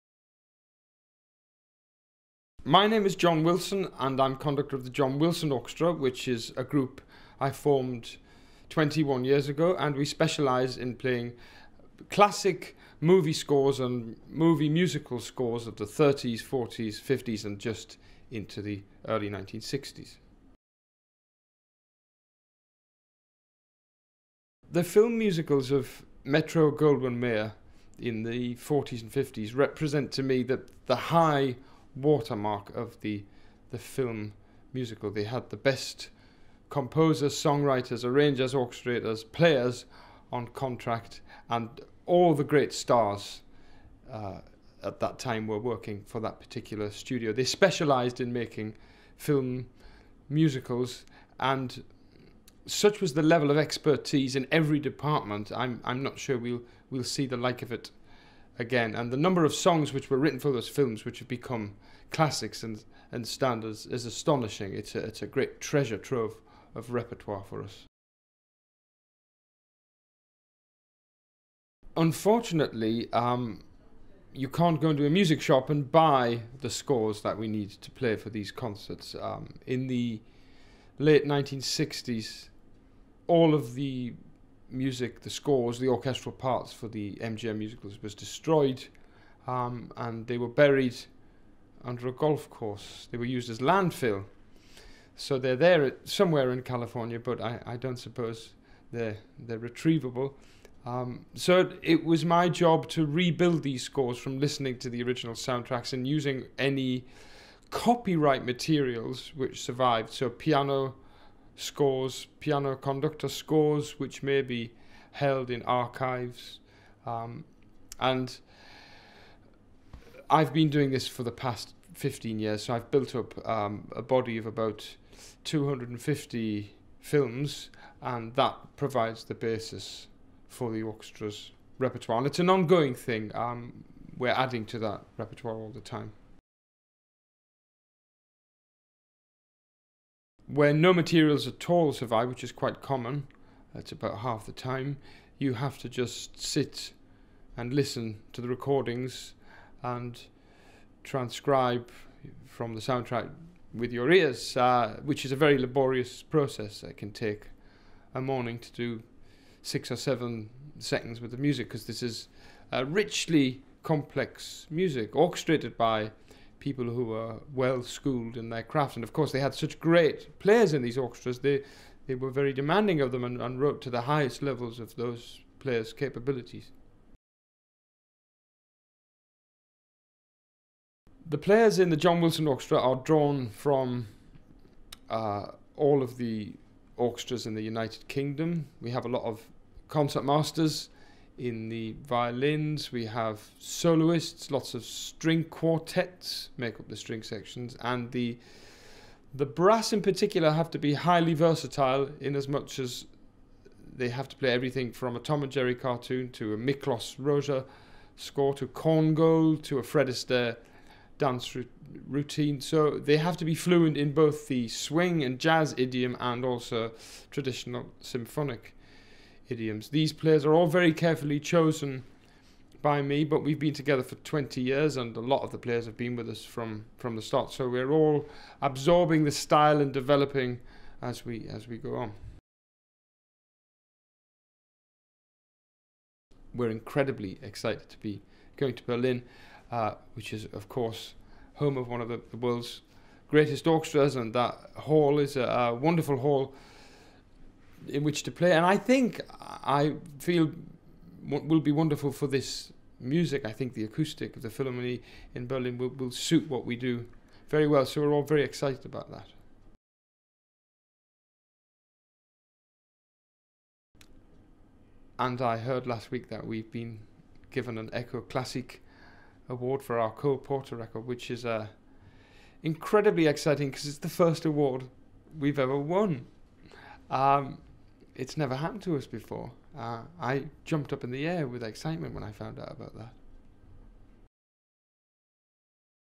Interview with John Wilson
In 2009, John Wilson and his John Wilson Orchestra celebrated the 75th jubilee of the Metro-Goldwyn-Mayer Film Musical at the Royal Albert Hall. Since then he has collaborated with the BBC Proms on an annual basis, with sold-out John Wilson concerts being broadcast around the world on both television and radio. John Wilson and the John Wilson Orchestra will now give their German debut at the 2016 Musikfest Berlin. Under the title “A Celebration of the MGM Film Musicals”, they present a select choice of the most popular titles from the golden 30s, 40s and 50s of Metro-Goldwyn-Mayer Film Musical productions. We asked Wilson about the relevance of film musicals and the difficulties of reconstructing the often lost music scores.